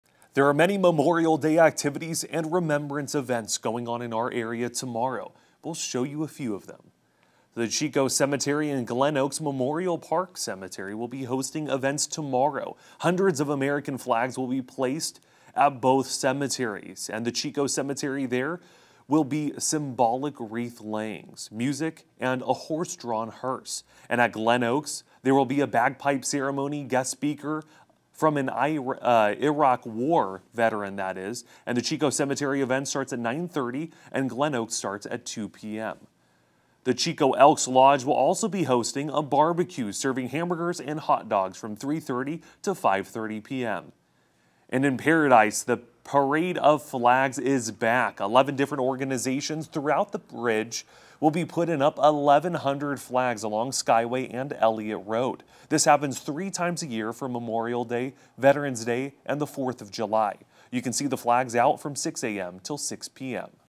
Multiple Memorial Day events to take place in Chico and Oroville Monday
Various Memorial Day events will be taking place in Chico, and the Parade of Flags will be taking place in Oroville.